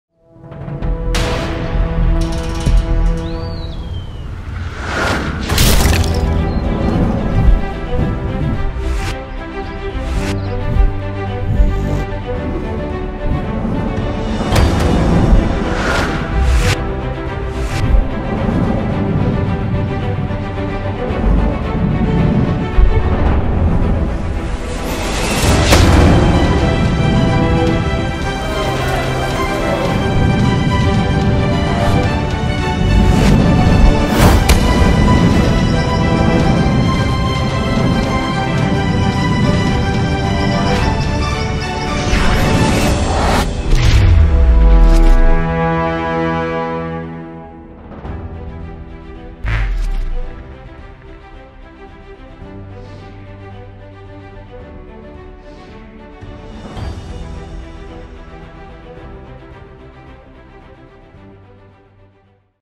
CAMPIONATI EUROPEI SCI ALPINISMO - Trofeo Internazionale dell'Etna - 22 | 23 | 24 Febbraio 2018
Promo ufficiale Campionati Europei Sci Alpinismo
Trofeo Internazionale dell'Etna - 22 | 23 | 24 Febbraio 2018